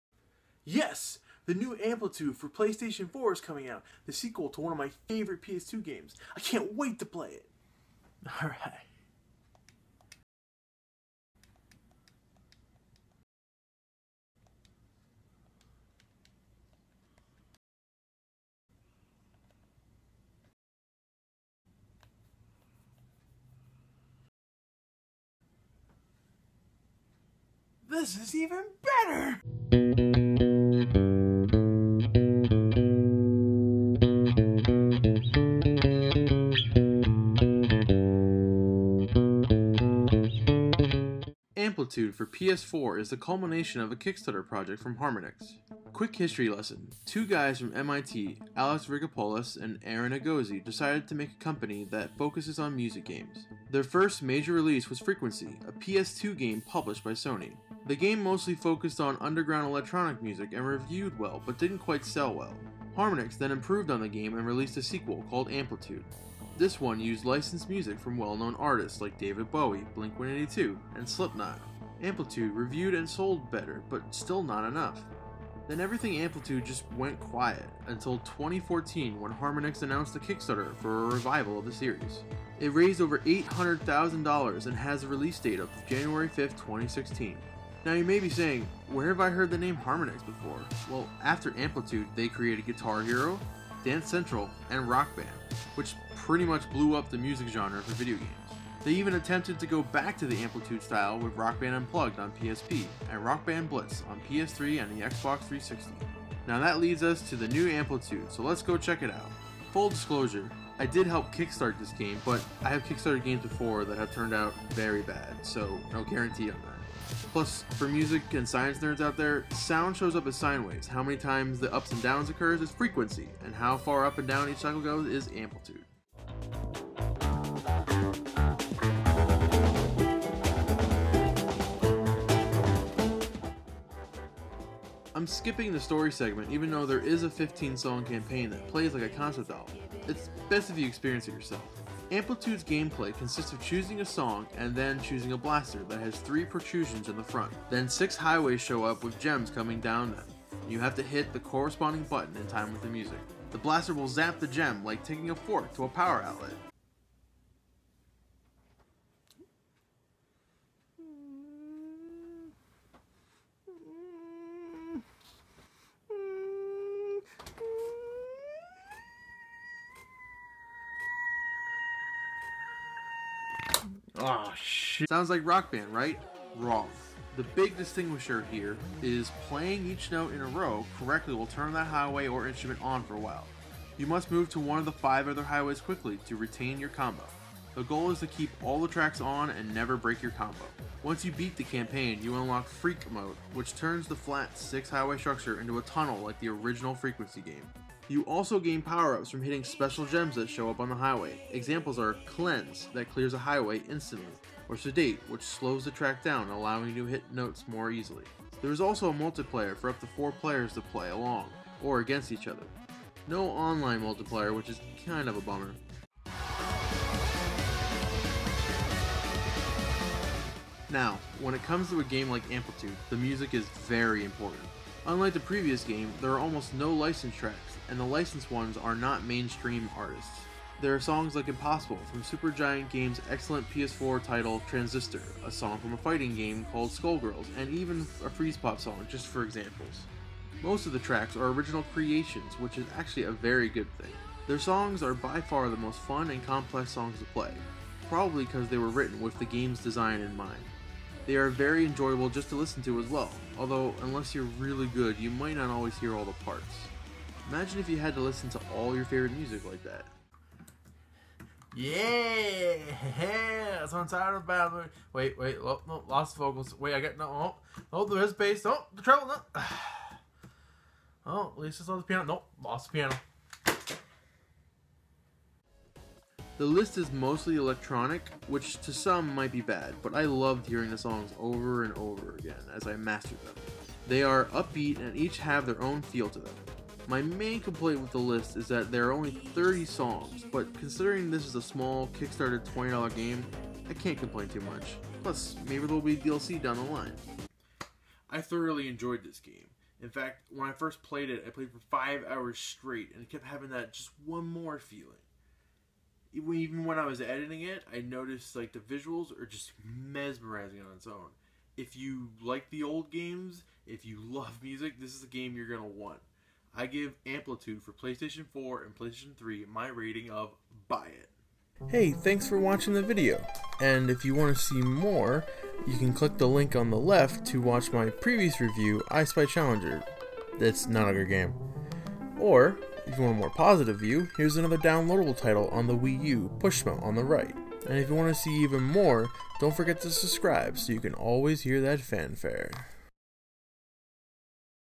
Amplitude (2016) Review
A review of the new game developed by Harmonix and kickstarted by the fans, Amplitude for PS4/PS3. A sequel to the two PS2 titles, FreQuency and Amplitude